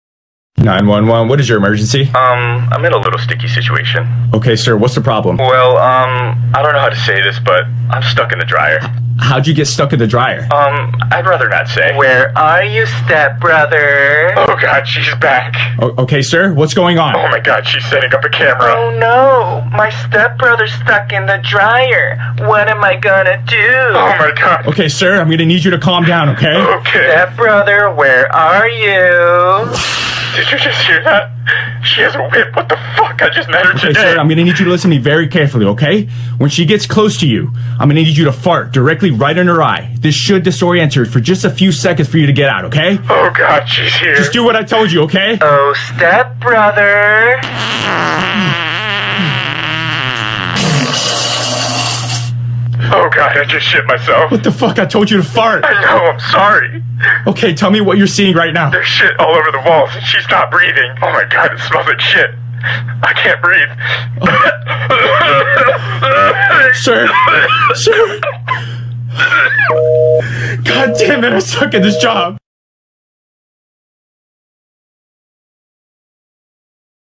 Worst 911 dispatcher ever 😂
Man calls 911 when his date goes wild😂 dispatcher gives horrible advice influencer25 #911